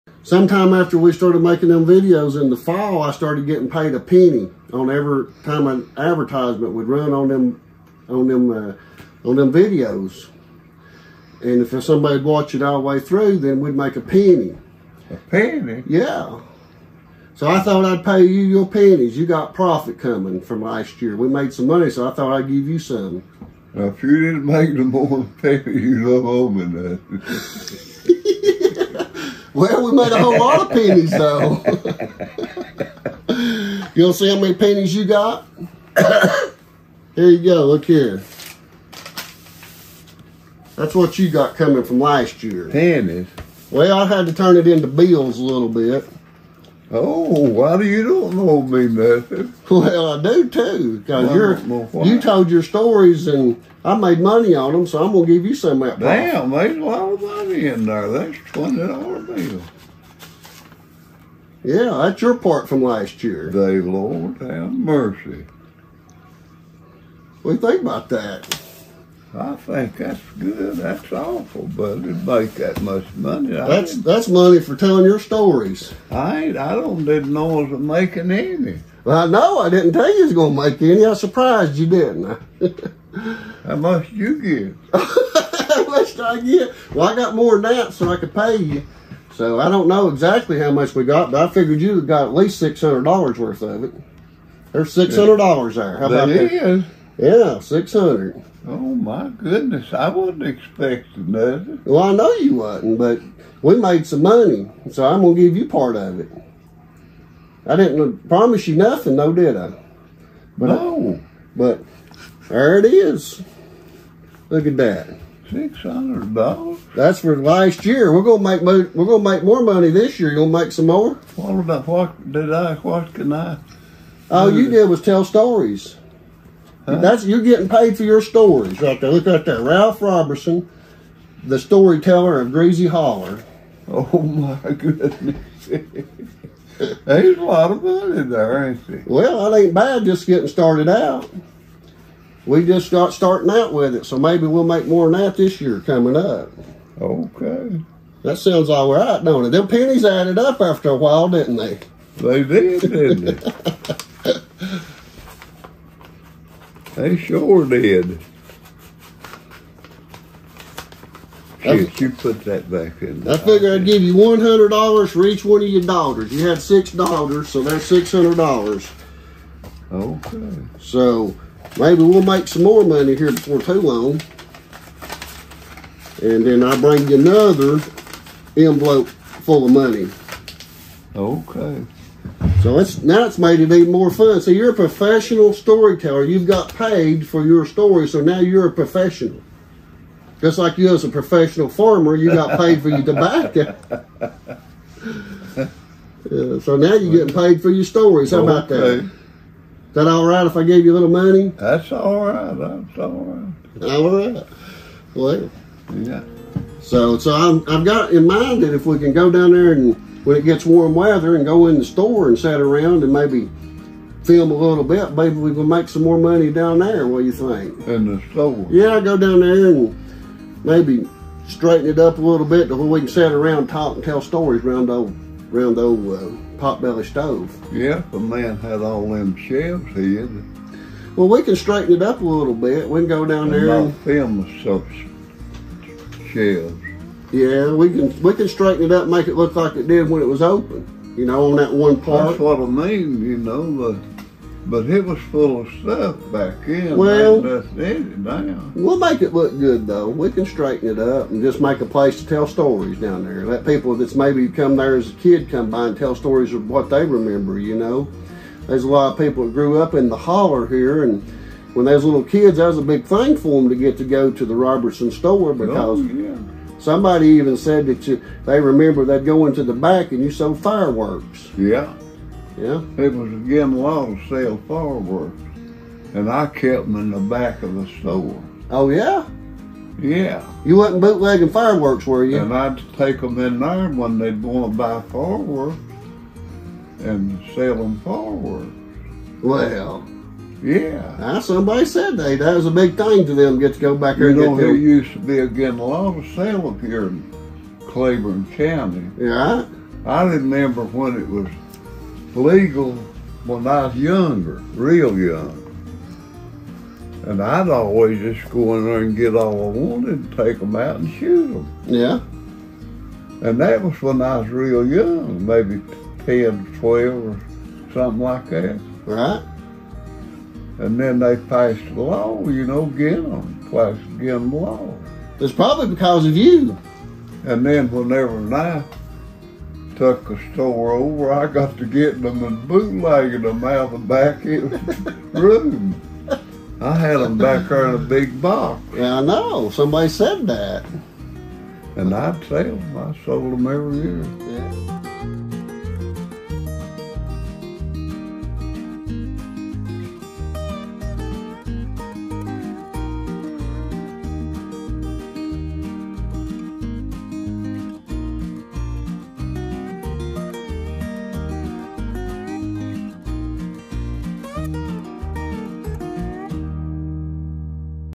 E18 95 Year Old Ralph Gets His Cut from Facebook Ad Revenue for telling his Stories
E18: In this episode Ralph becomes a professional storyteller when he gets his cut from my Facebook ad revenue.